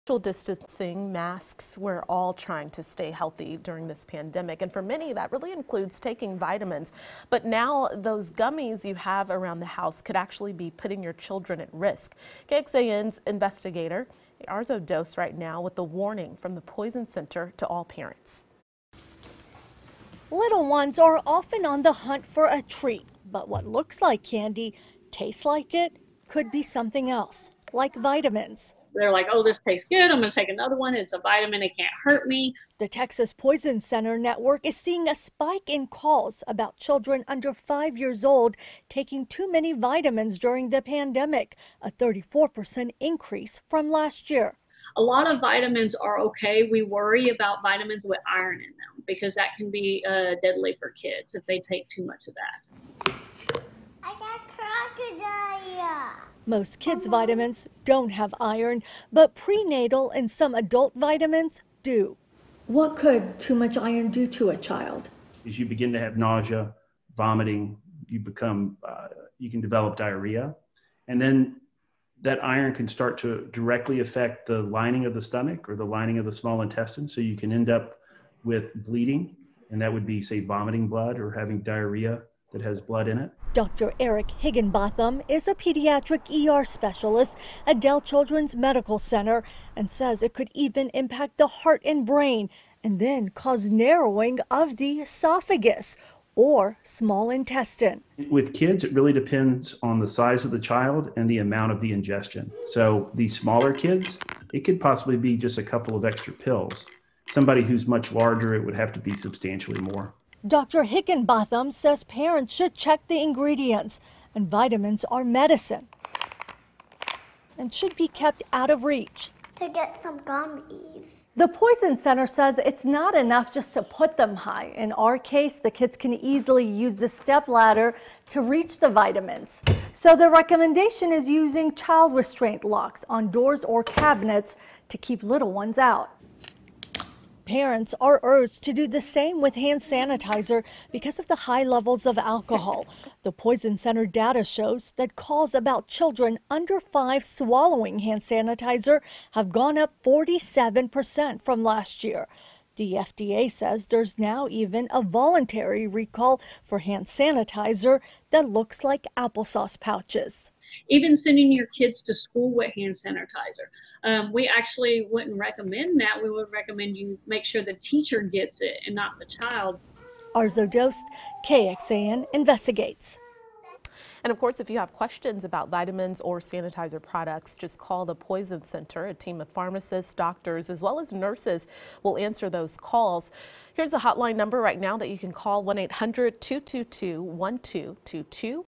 Are kids taking too many vitamins? Texas Poison Center reports spike in calls
KXAN Investigator Arezow Doost reports.